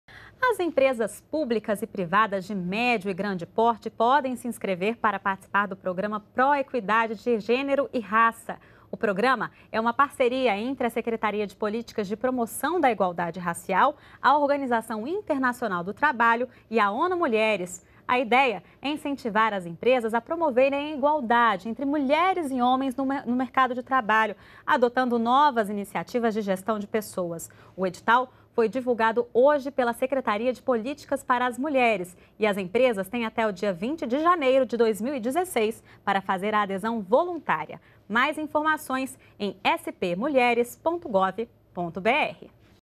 Empresas já podem fazer adesão voluntária ao programa Pró-Equidade de Gênero e Raça
NBR NOTÍCIAS - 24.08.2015: As empresas públicas e privadas de médio e grande porte podem se inscrever para participar do programa Pró-Equidade de Gênero e Raça, da Secretaria de Políticas de Promoção da Igualdade Racial, em parceria com a Organização Internacional do Trabalho e a Onu Mulheres. O edital foi divulgado nesta segunda-feira (24) pela Secretaria de Políticas para as Mulheres e as empresas têm até 20 de janeiro de 2016 para fazer a adesão voluntária. Mais informações em spmulheres.gov.br